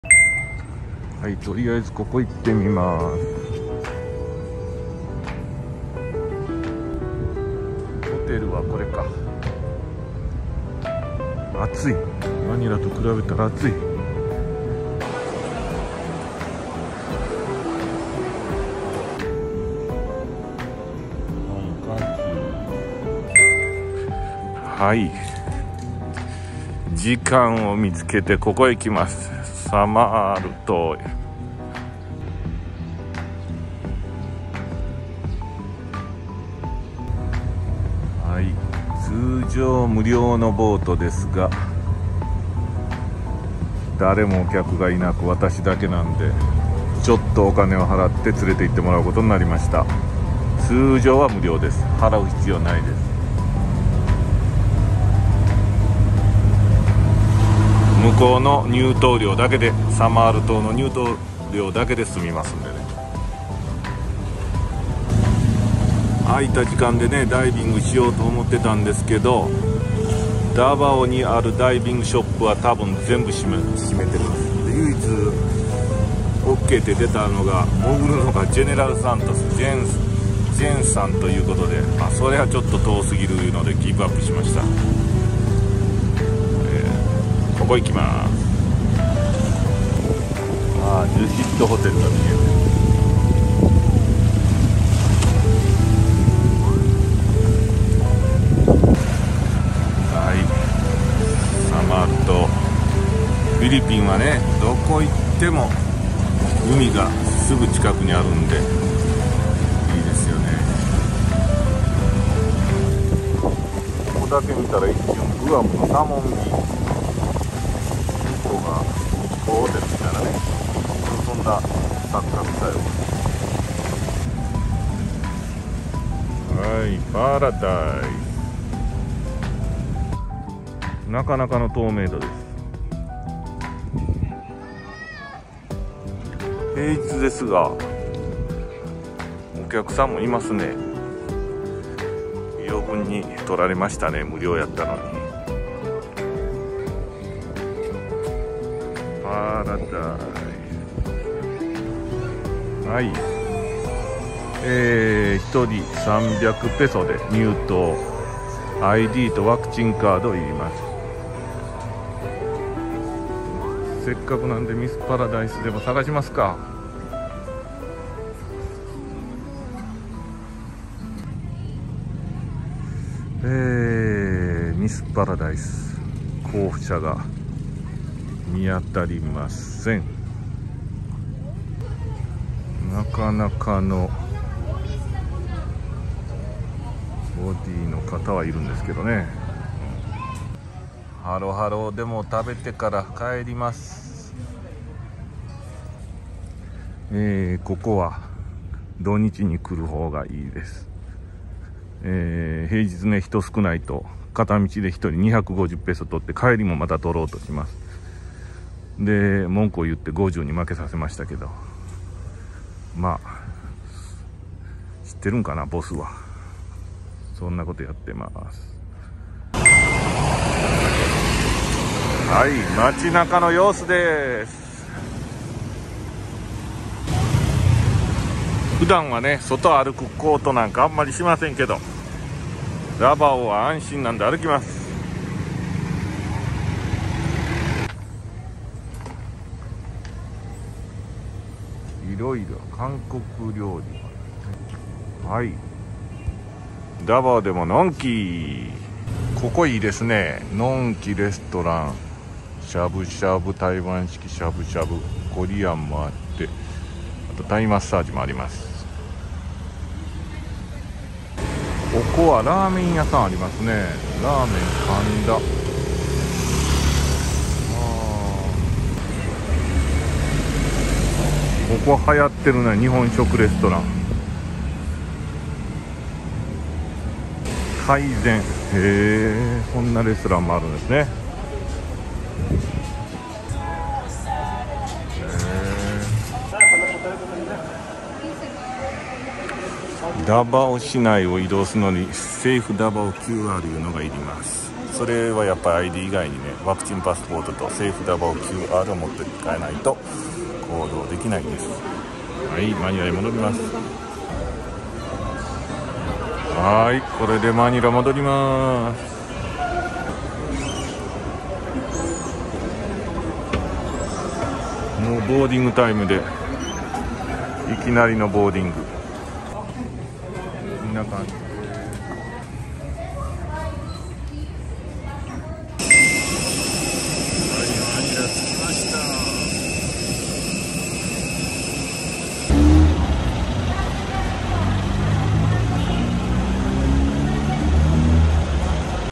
Dec.2〜3, 2021 🇵🇭 Davao short trip. Sea & Town. ダバオでの様子。街と海。
2021年12月2~3日
ダバオでの行動記録の様子です。
ダイビングショップはたぶん全部閉鎖中。
リカーバンでお酒もまだ飲めません。
KTVも閉鎖中でした。
ダバオ市内の行動では、Safe Davao QR登録が必要です。
マニラに比べて、めちゃくちゃ暑く、日本の夏に本土から沖縄に来た感じでした。
12月や1月は、セブやダバオが良いかもと思いました。